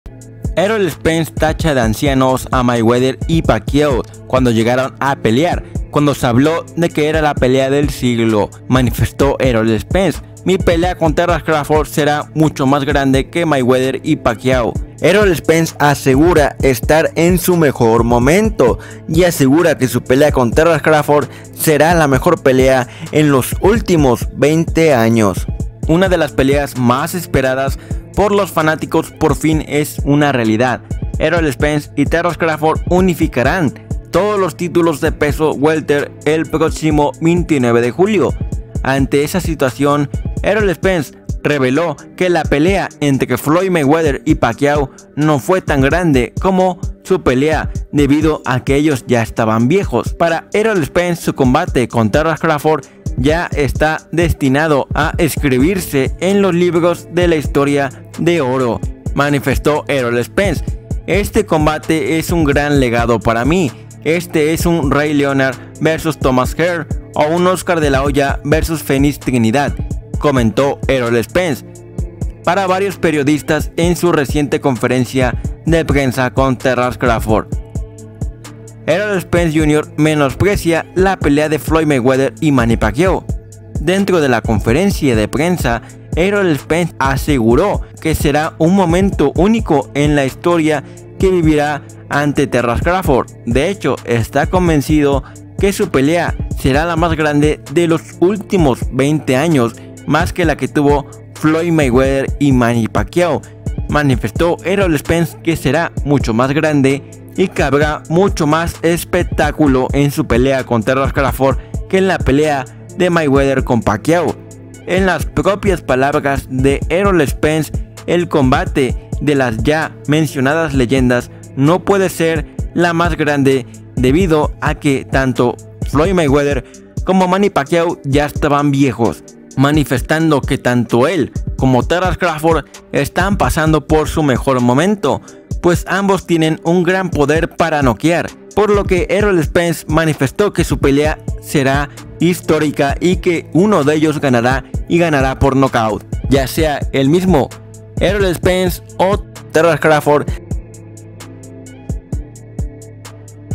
ERROL SPENCE se BURLA de MAYWEATHER y PACQUIAO pelearon ancianos TERENCE CRAWFORD Y YO NO
ERROL SPENCE CRITICA A FLOYD MAWEATHER Y MANNY PACQUIAO!🥊
ERROL SPENCE ASEGURA QUE SU PELEA CONTRA TERENCE CRAWFORD SERA SUPERIOR ALA DE MAYWEATHER Y PACQUIAO POR EL ECHO DE QUE ELLOS YA ESTABAN VIEJOS CUANDO PELEARON ERROL SPENCE ASEGURA QUE SU PELEA CON TERENCE CRAWFORD SERA LA VERDADERA PELEA DEL SIGLO!